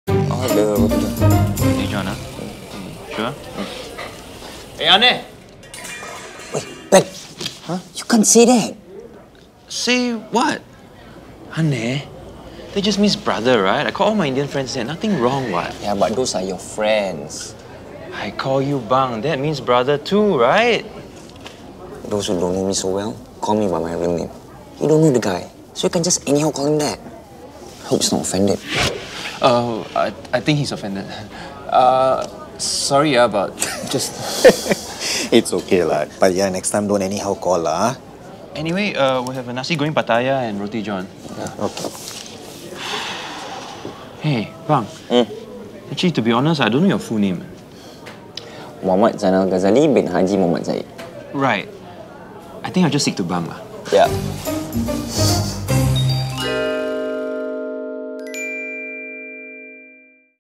Ben & Bang - Ordering Food
Ben & Bang argue about what to call the local 'mamak'. Who's right and who's wrong?